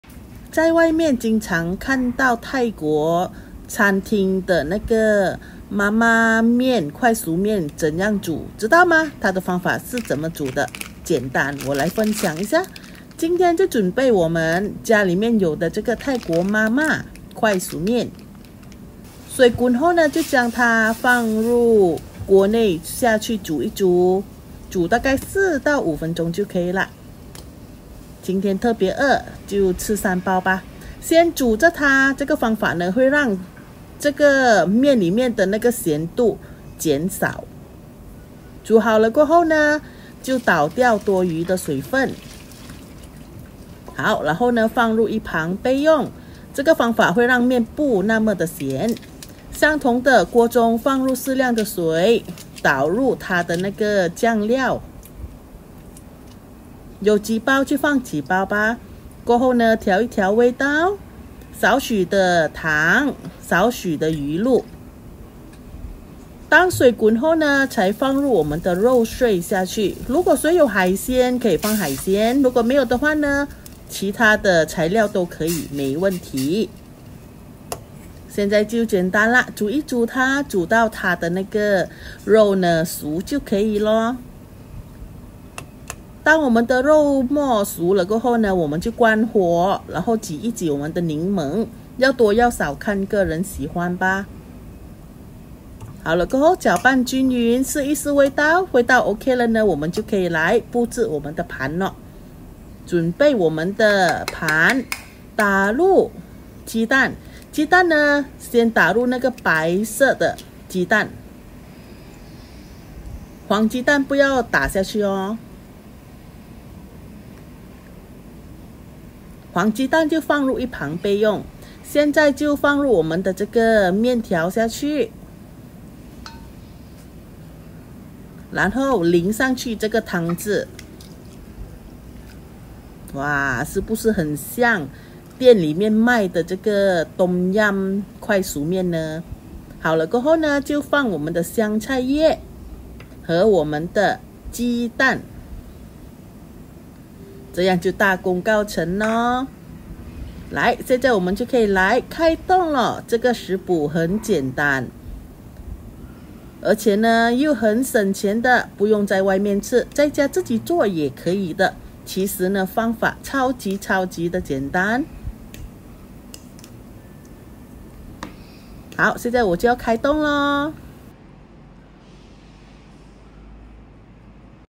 原来泰国餐厅的妈妈快熟面是这样煮的！ 学起来 好吃有省钱！
材料
泰国妈妈冬阴快熟面 3包
香菜叶
1 汤匙白糖
1 茶匙 鱼露
1粒 青柠檬
1 L 水
一个鸡蛋
300克 肉末